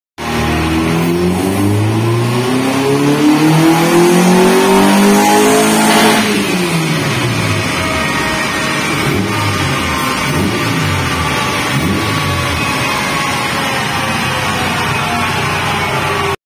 Honda b20b in for repairs and tuning no before results after the tune made 110kws 201nm
We did injectors intake swap plugs and tuning